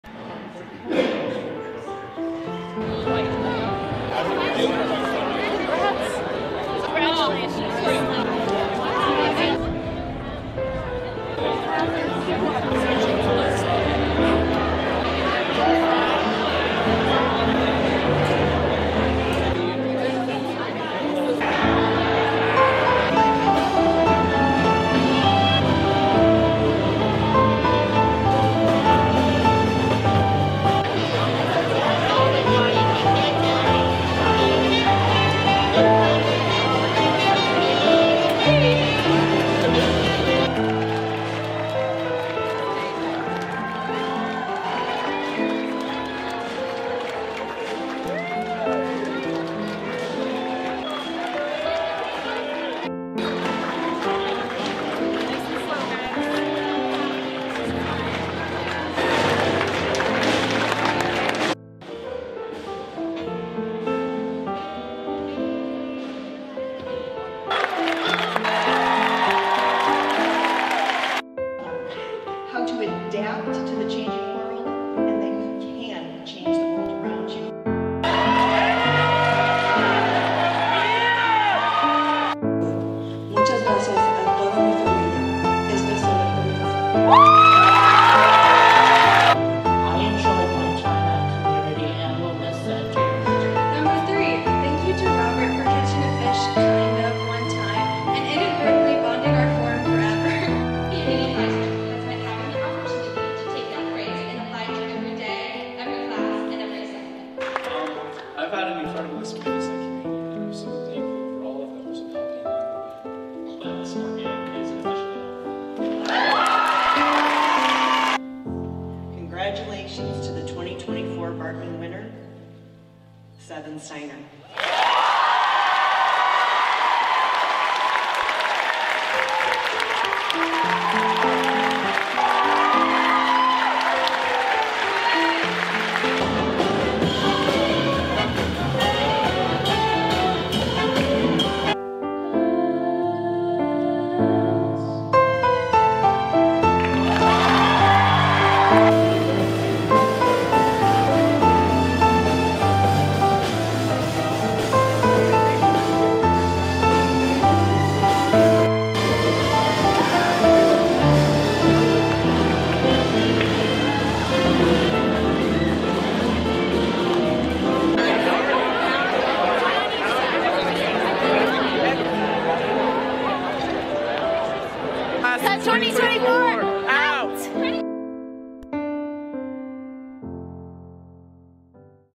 CHS Graduation 2024
The class of 2024 has their end of the year graduation at Hill auditorium.